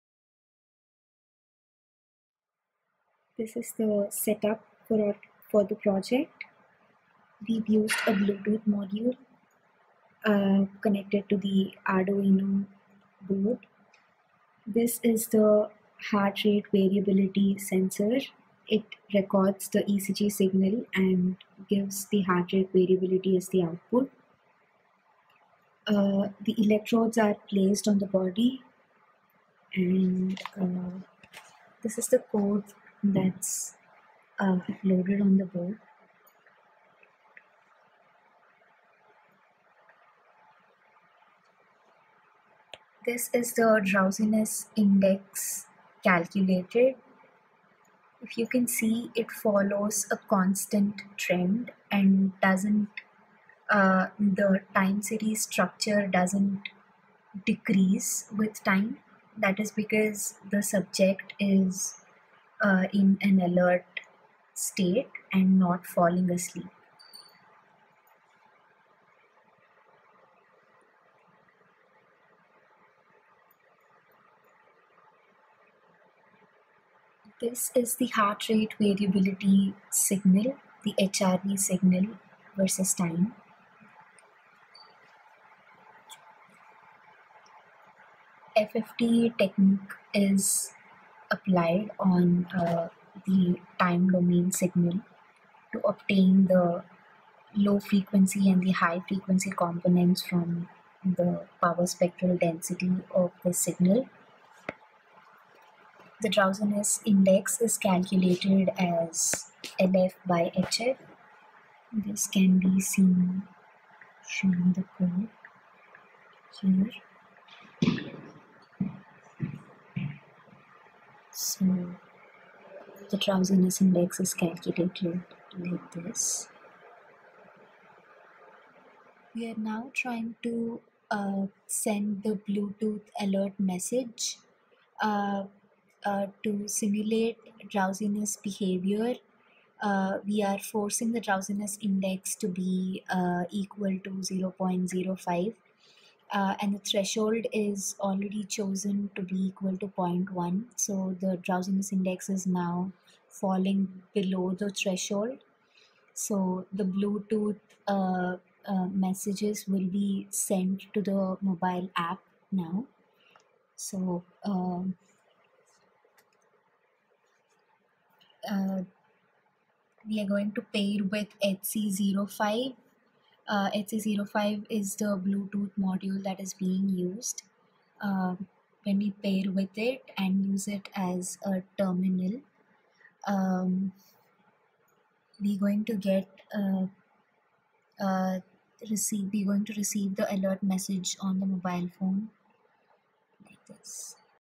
Vehicle Driver Drowsiness detection using ECG signal
Used Arduino UNO, AD8232 heart-rate monitor and HC-10 bluetooth transceiver as sensor node and android application on a mobile phone as sink node for heart-rate analysis for detection of various levels of drowsiness from the ECG signal of a vehicle’s driver and send an alert in case of drowsy driving.